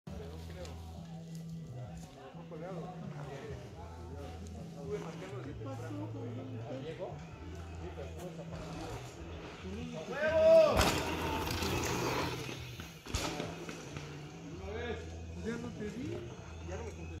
A volar con las Diamondback Release.
Nuestro Amigo Jake poniendo a prueba y haciendo volar a la Poderosa Diamondback Release 2.

A tope Campeónes.

¡Que esperas a venir por la Tuya!

¡Tenemos el crédito que tú necesitas!

Pregunta por nuestros Modelos

*Aplica para mayores de 20 años